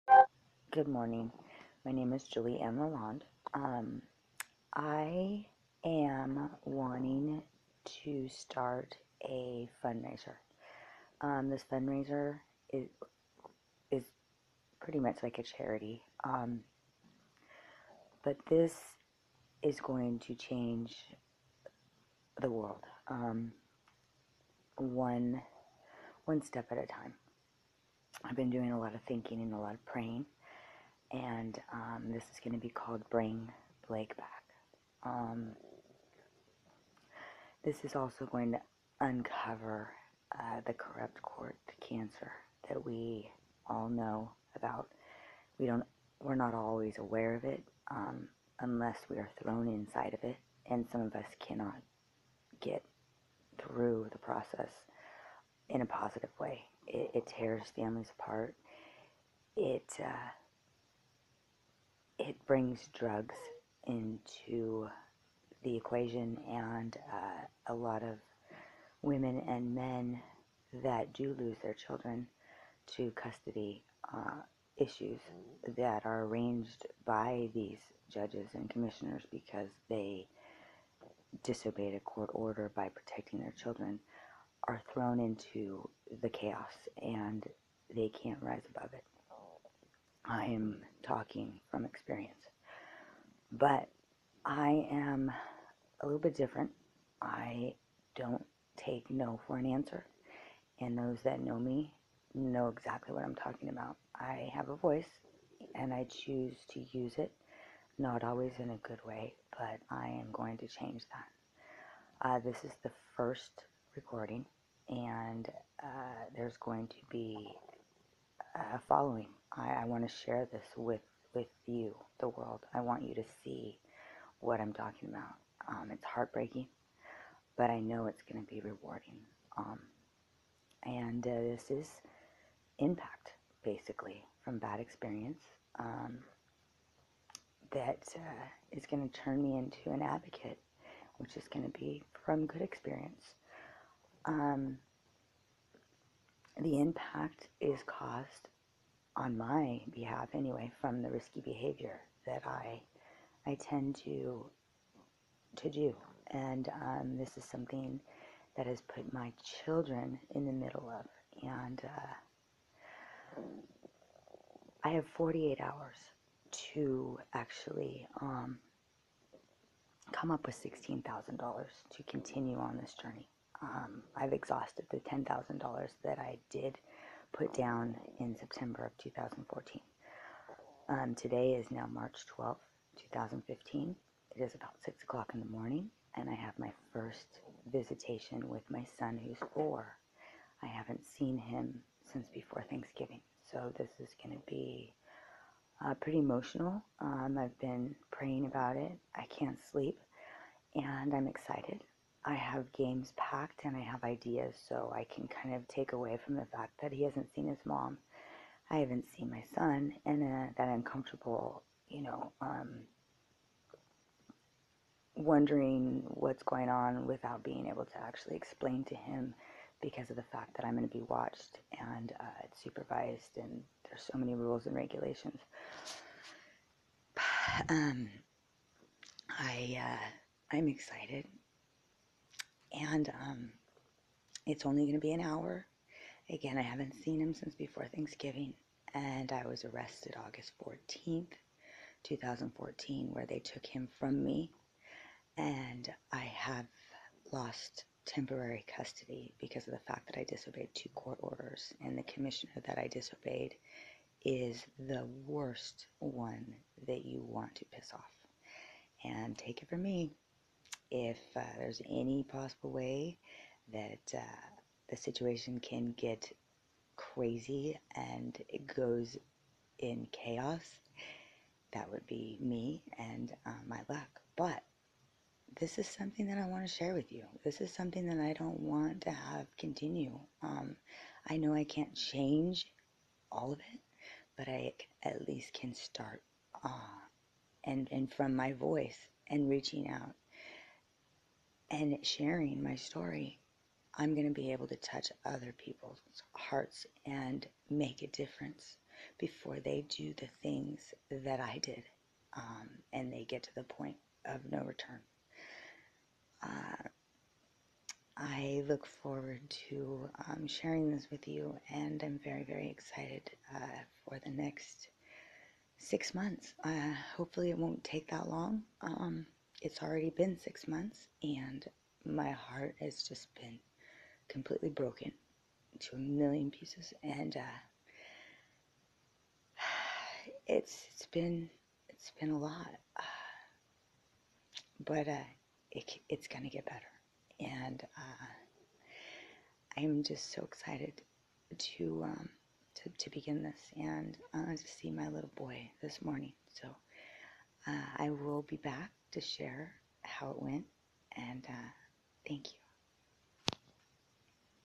LEGALLY KIDNAPPED!!!!! 2014 They Stole my toddler discredited any concerns or his confession
This was the very beginning. HAHAHA , Like I thought this could never happen.
This is one of many letters' I wrote to the DA, NO ONE CARES!
Dear Mr.

I left you a voicemail last week regarding a very serious matter, my 4 year old son who is currently living full-time with his father.
This is all because I tried to get help from our court system here in Sacramento County- and as a protective mother I did not turn him over to his Father as I was told to by our commissioner twice. I spent the night in jail and the charges were dismissed. This story gets a little more intense. BUT THE MAIN CONCERN IS MY SON who is in serious danger and being abused every day.
I have no contact with him, my lawyer dumped me when the money ran out and he now has full custody and I have no forwarding address...

Please read my attached letter, The press release, & recordings of my son admitting the horror of what and where he is constantly being abused.